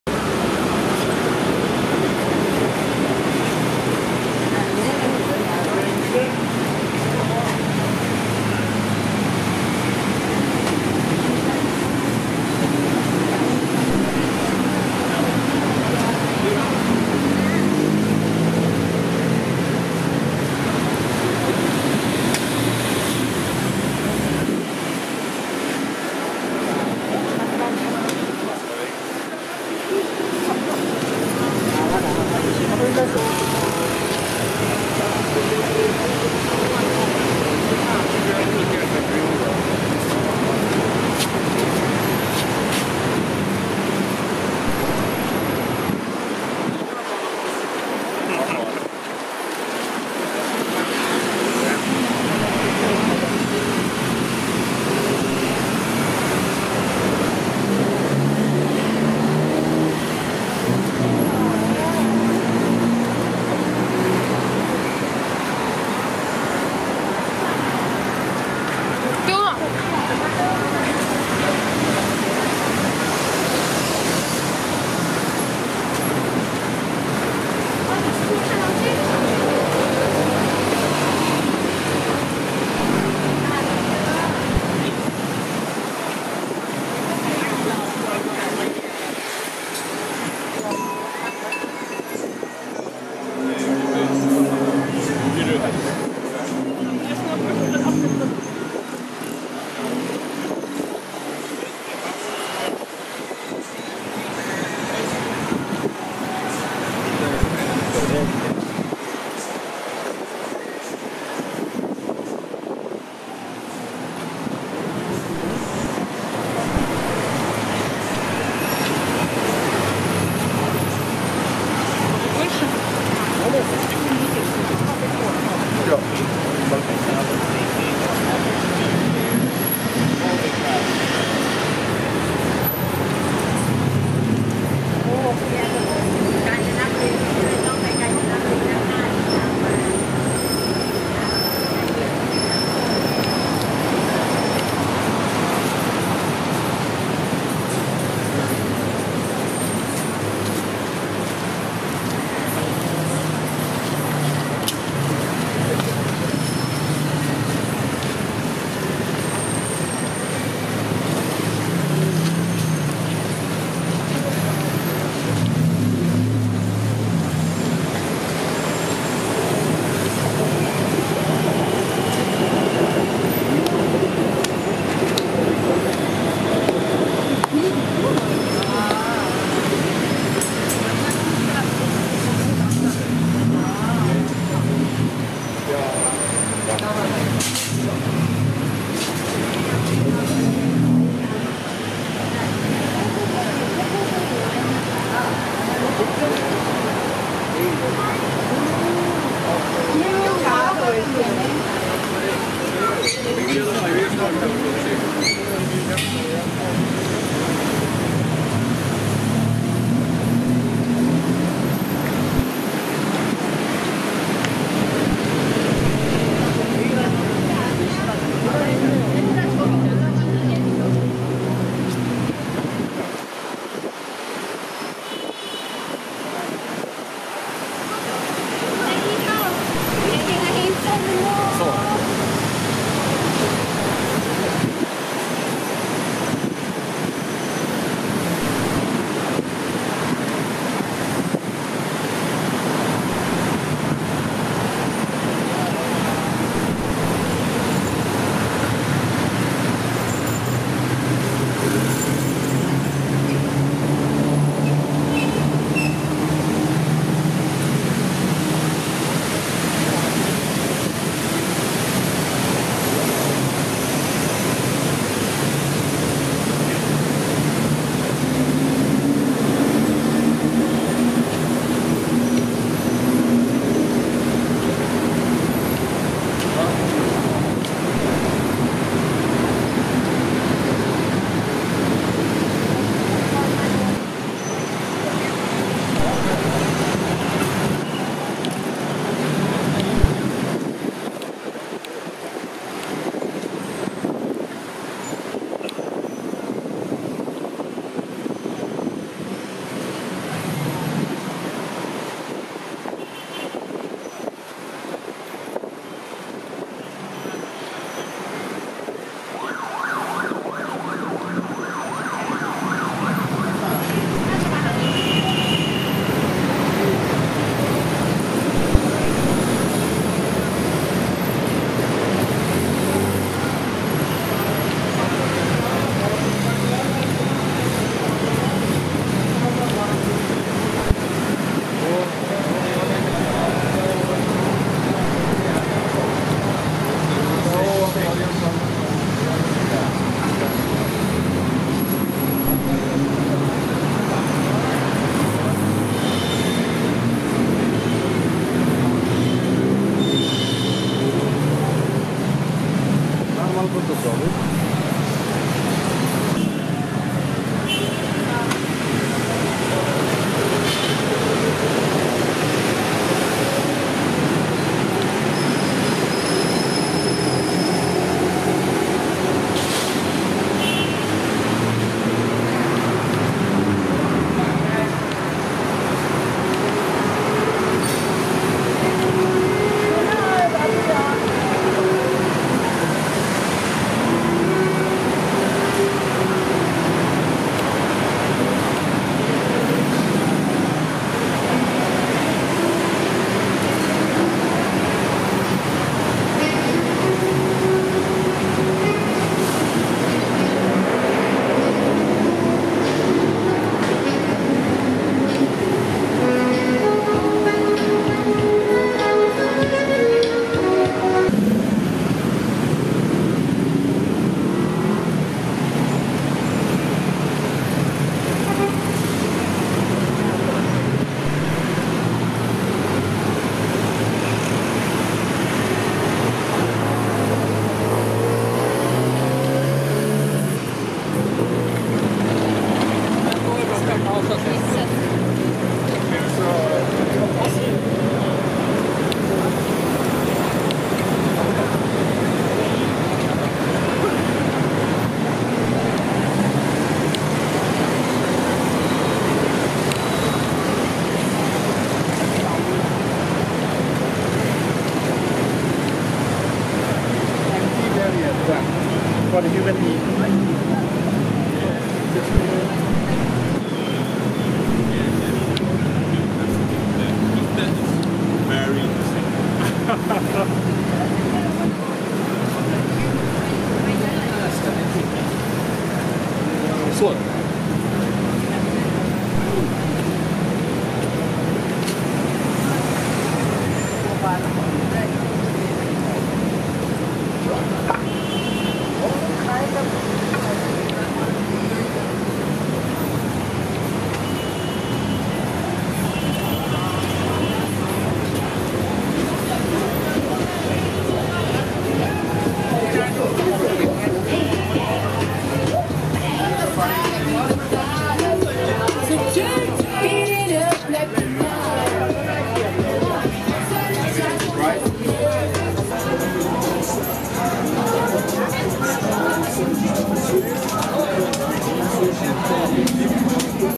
Sukhumvit Night Walk to Soi Cowboy - Bangkok 2019
A Night Walk along Sukhumvit Road to popular Soi Cowboy. One of Bangkoks most popular Nightlife and Red Light Districts. The Area is alive Day and Night